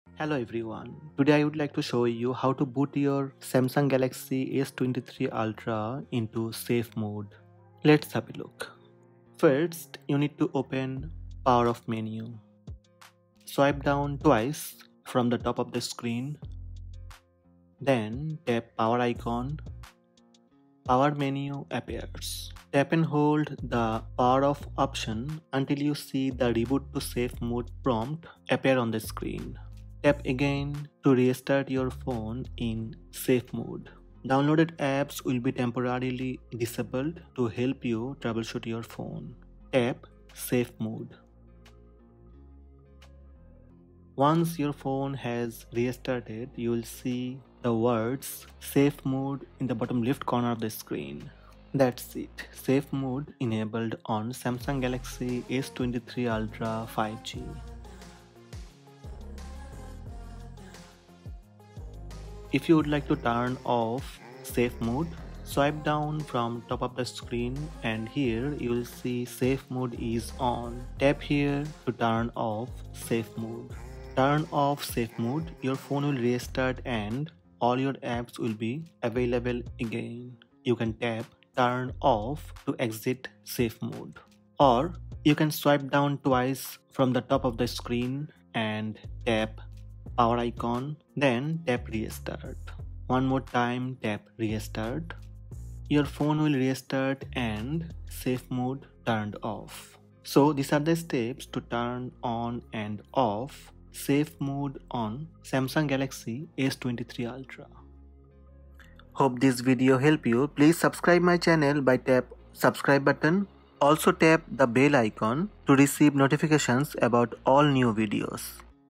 How to Boot your Samsung Galaxy S23 Ultra into Safe Mode
(1:00) How to turn off safe mode on  Samsung Galaxy S23 Ultra.